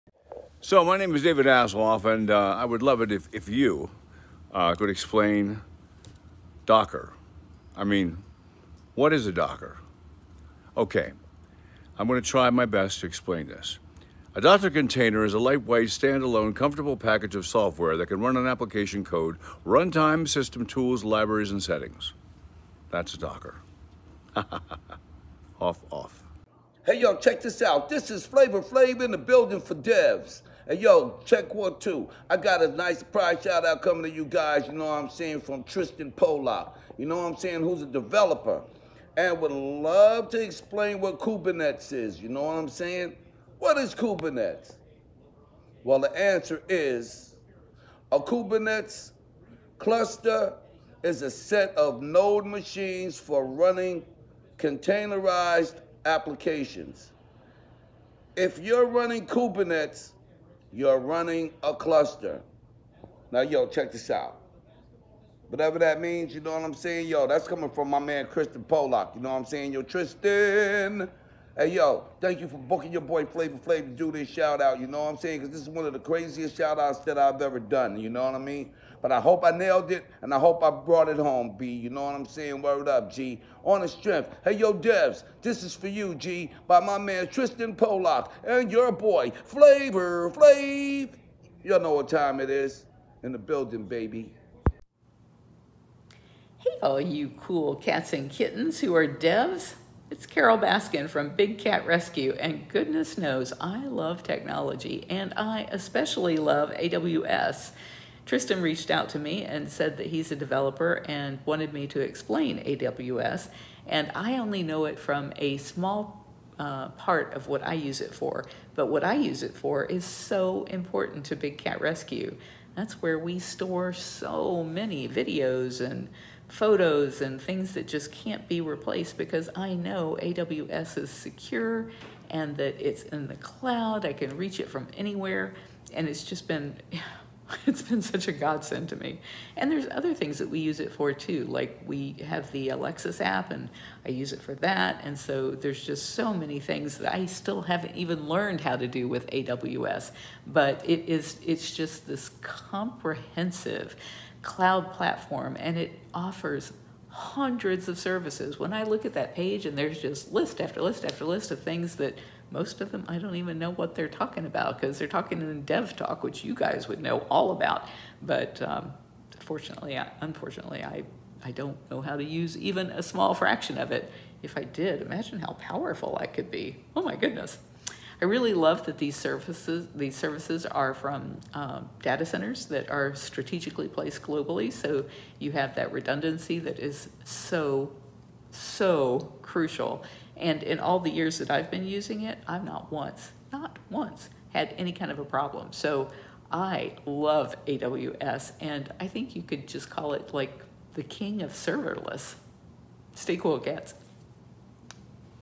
Celebrities Explain DevOps
David Hasselhoff, Flavor Flav, and Carole Baskin, help simplify AWS, Kubernetes, and Docker. Yes, they actually do.

// About CTO.ai //
CTO.ai is a developer workflow platform built to increase the simplicity, velocity, and observability of your dev team in CLI + Slack.